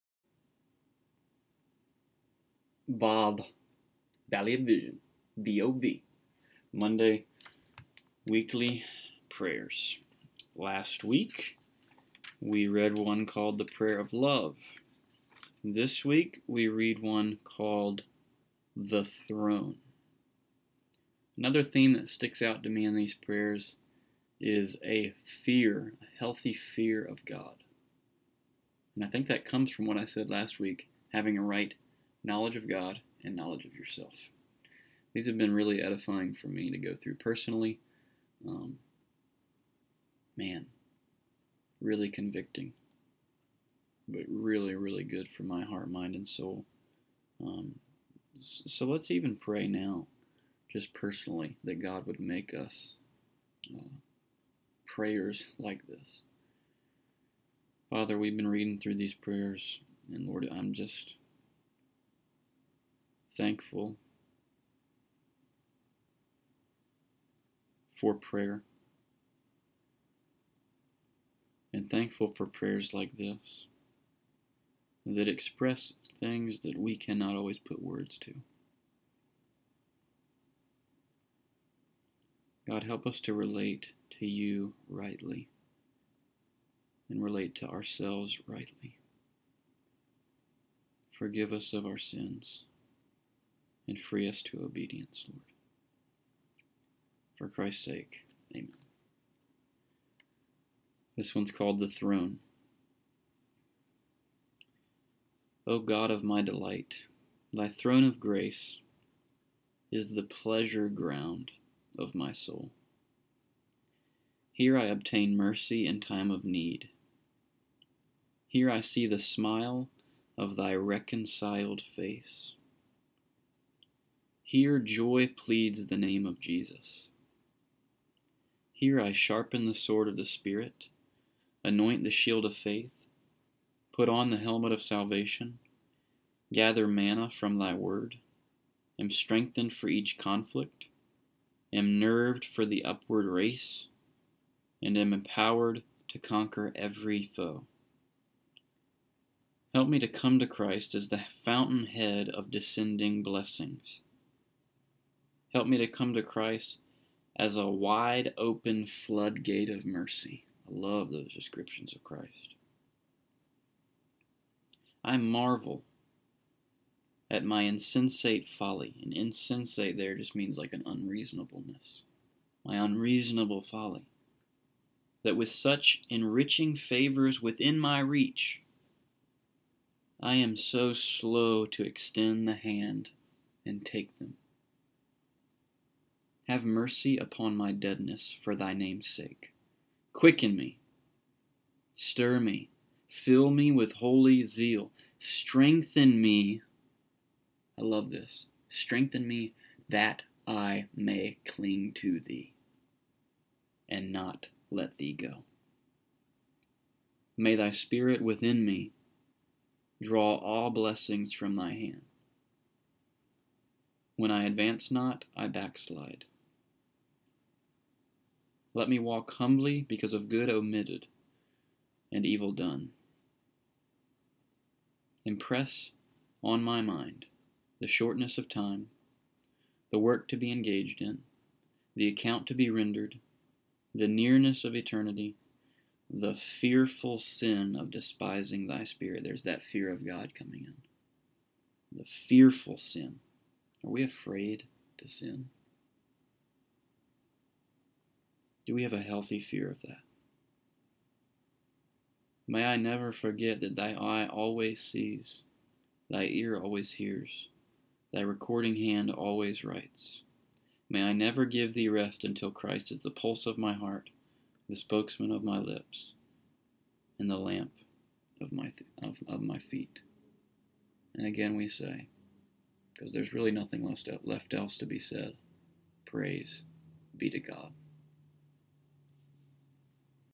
Valley of Vision Weekly Prayers: "The Throne"
Every Monday for 9 weeks we will be working through a short prayer from the Valley of Vision. This week we read on called "The Throne."

Join us as we pray together!